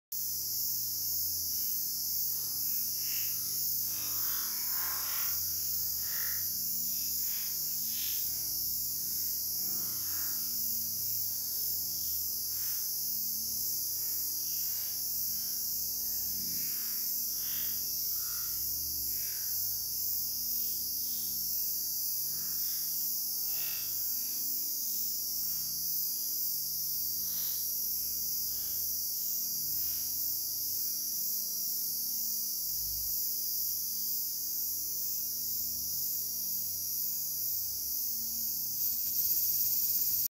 アヒルの食べ方
クチバシの中に餌と水を入れ、咀嚼しながら横から水だけ出しています。